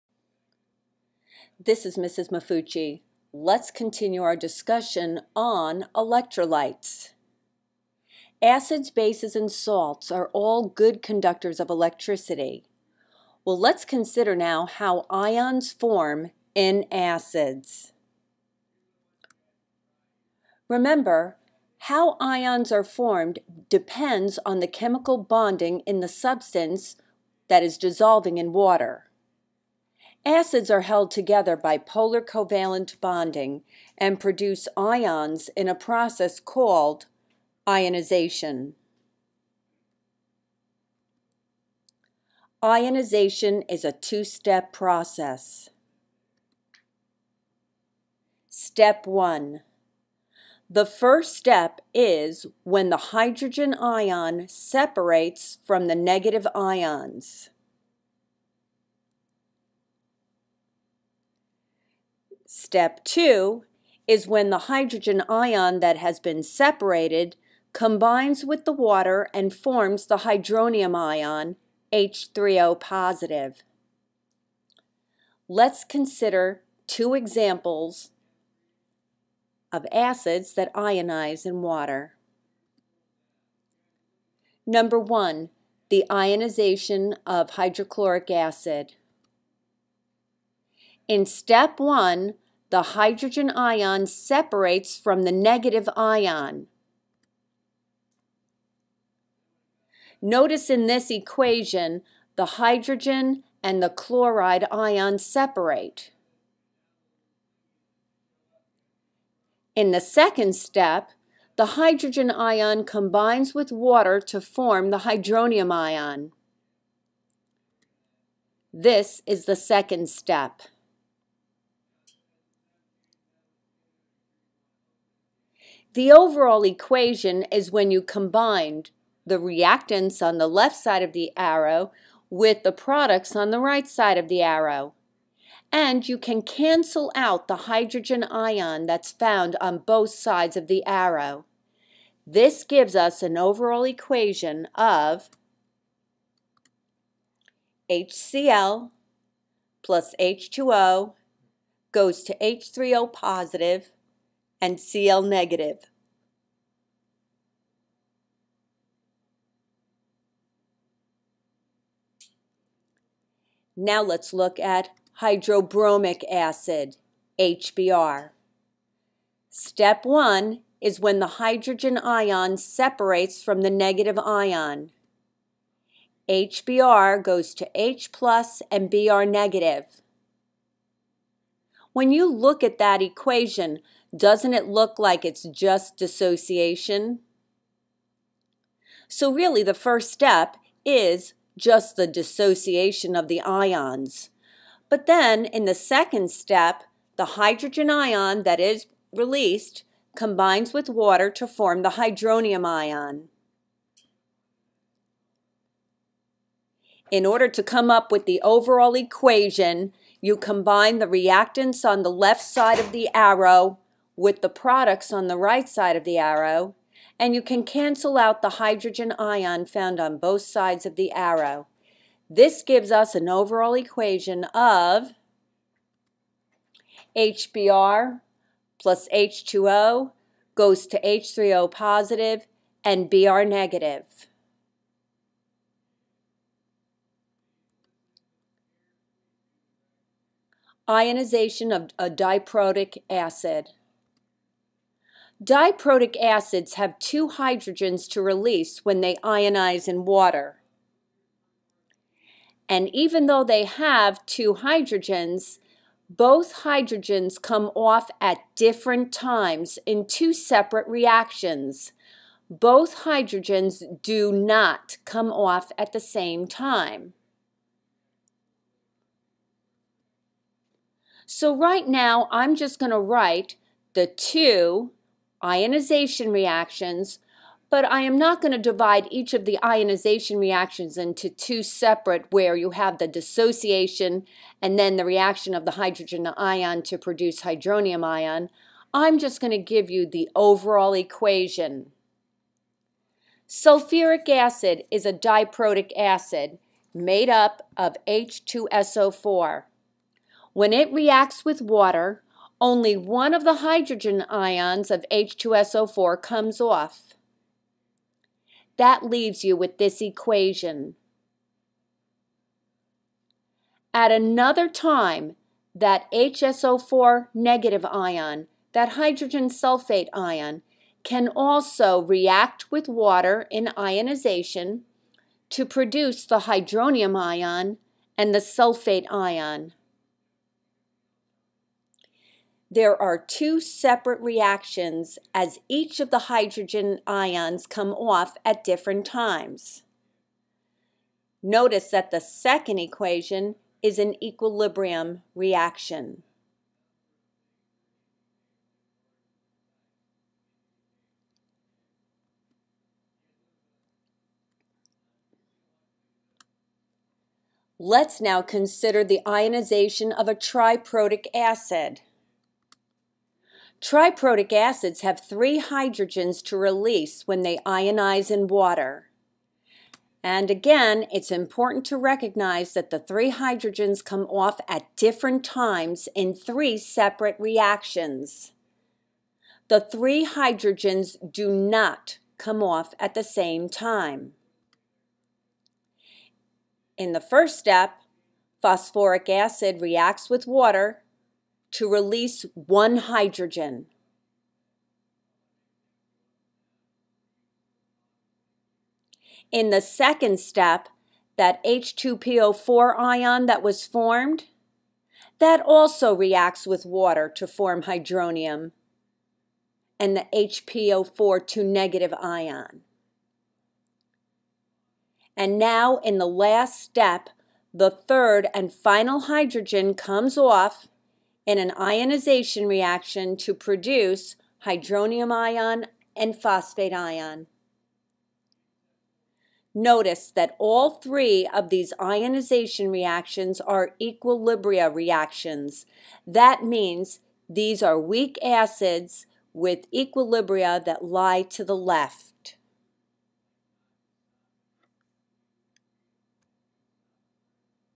Ionization of Acids
How to write the ionization of monoprotic, diprotic, and triprotic acids